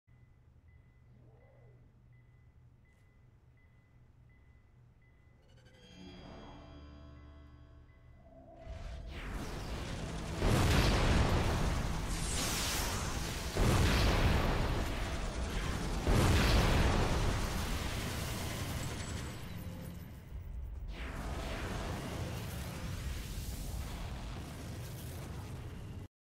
Changling Warshade burst combo
Drops 54 group in 12 seconds.
Nova Det - Nova Flight - Dwarf Mire - Eclipse - Sunless - Nova Det - Dwarf Mire - Quasar